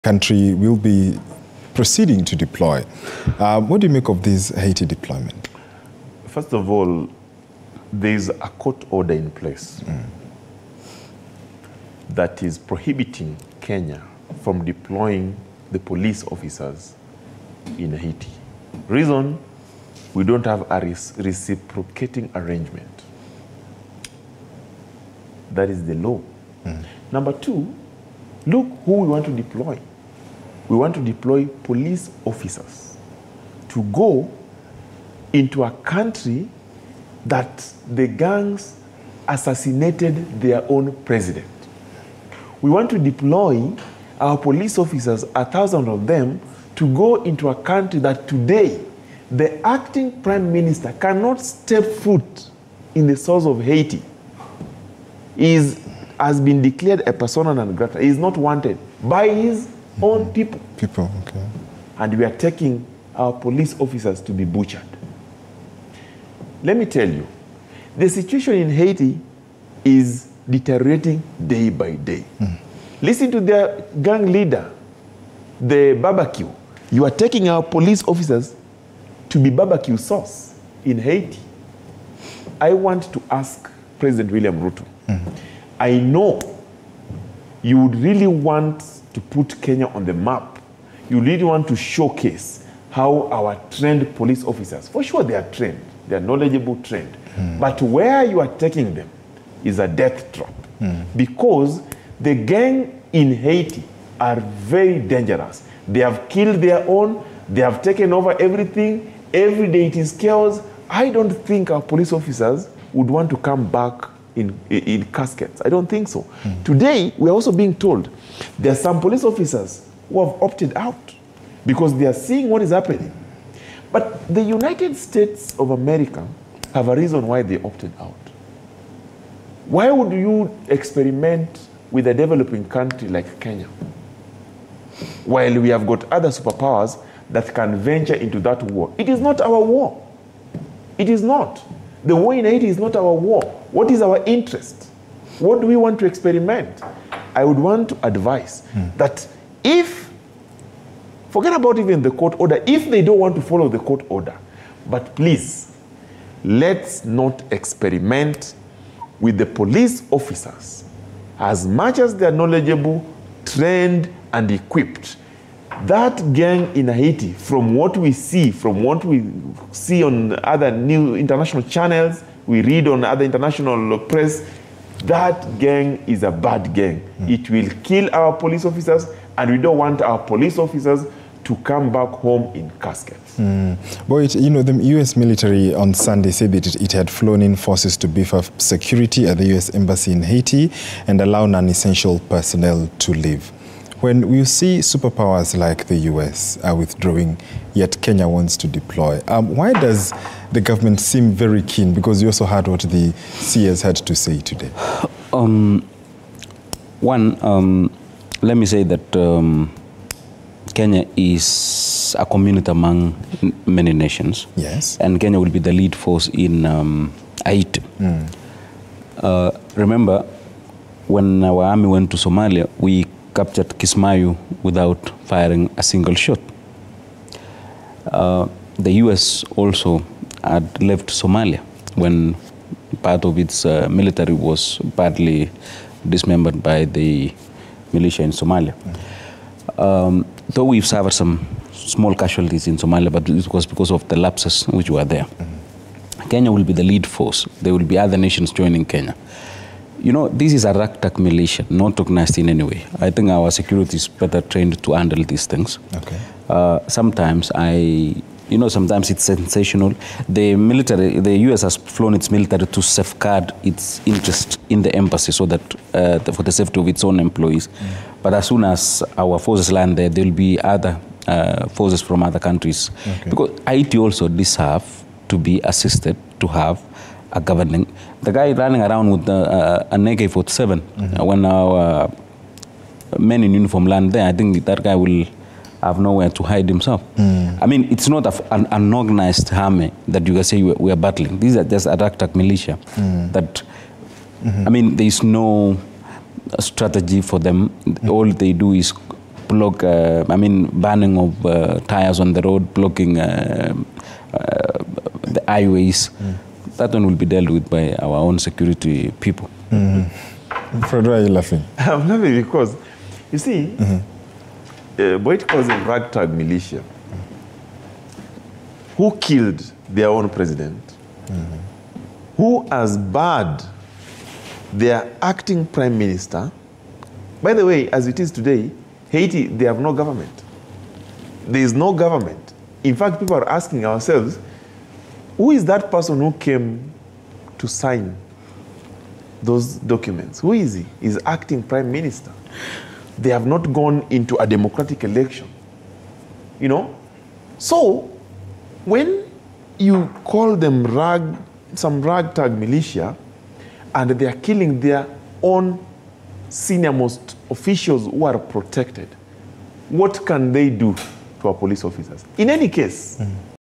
The war in Haiti is not our war: Fred Okango
The war in Haiti is not our war. What do we have to gain? Let us not experiment with our police officers. That gang (in Haiti) is a bad gang. - Fred Okango, Political Affairs Sec, Kanu.